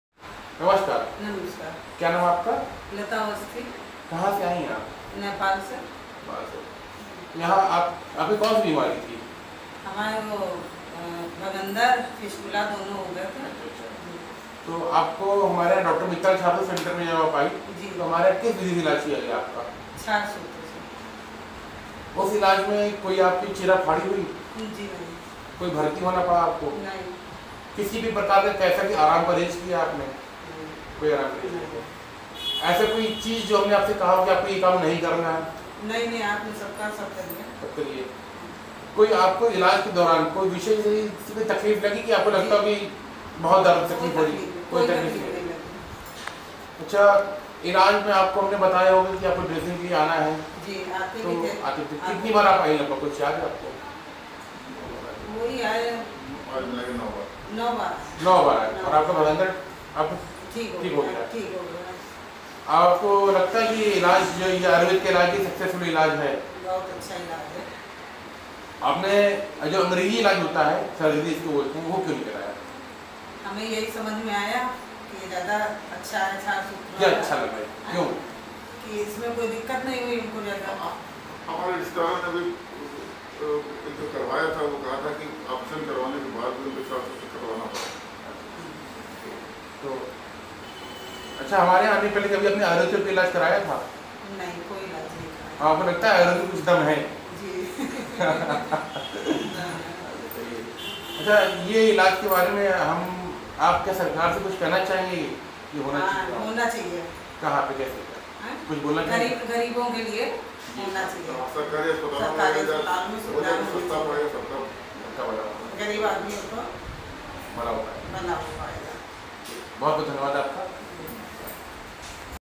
Anal Fistula by Kshar Sutra
patient Mrs Lata Awasthi came from Nepal for the treatment of her anal fissure and anal fistula.
shee was very satisfied after Ayurvedic Kshar Sutra ligation treatment because there was no pain or precaution.
her husband shared the experience of a patient who had surgery which was filled and had been undergone Kshar Sutra ligation therapy for recovery.

no cut
no incision
no anaesthesia
no western Chemical Synthetic Drugs
no western Chemical Antibiotics
no analgesic
no rest
no precautions
no Hospitalization

Ayurvedic Kshar Sutra ligation therapy